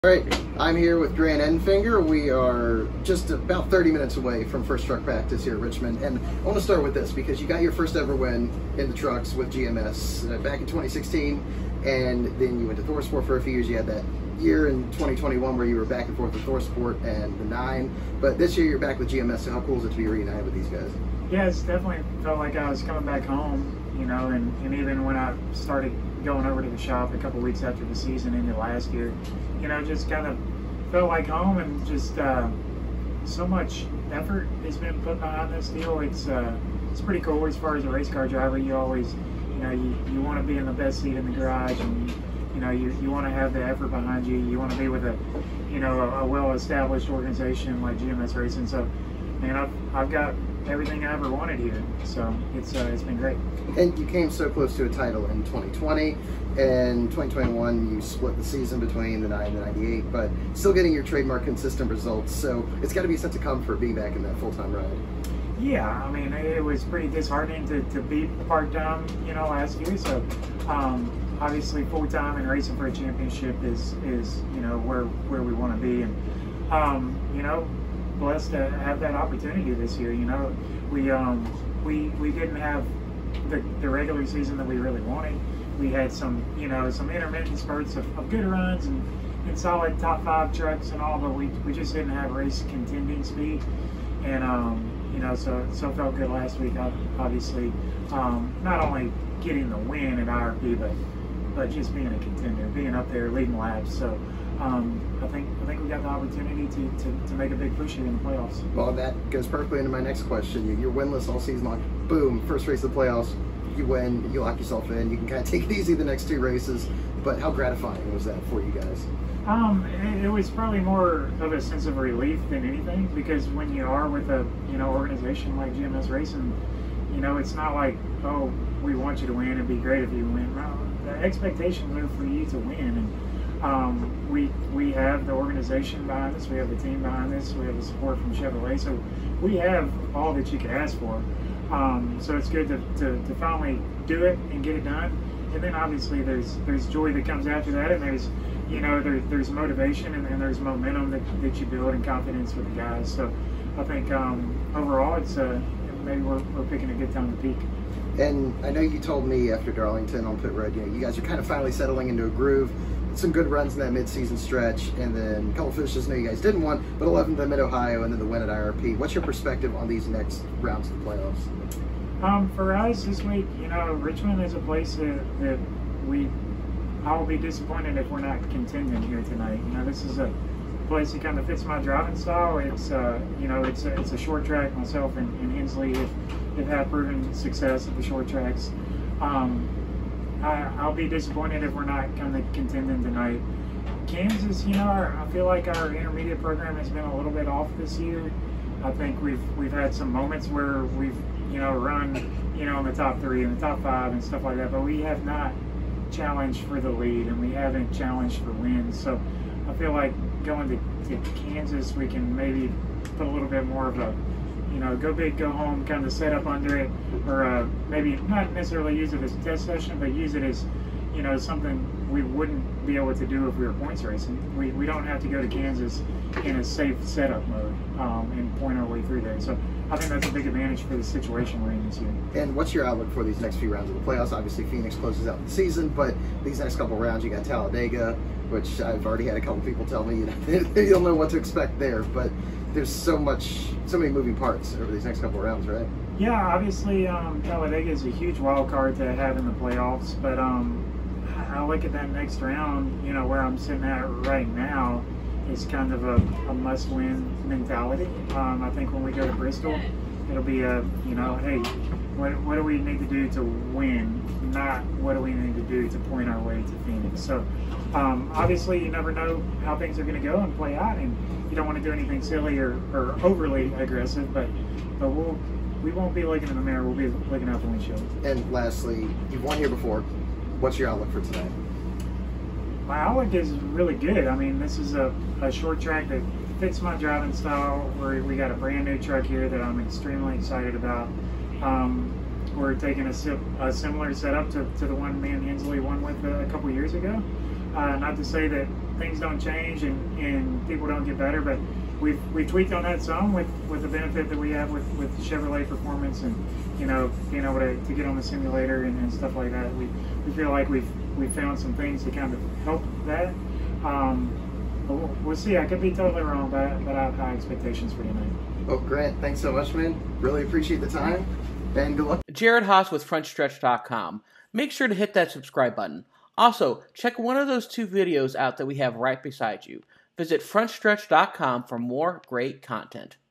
Grant Enfinger on Returning to GMS Racing and Making the Playoffs
Grant Enfinger sits down with Frontstretch's Adam Cheek to talk about his 2022 NASCAR Camping World Truck Series season and returning to GMS Racing.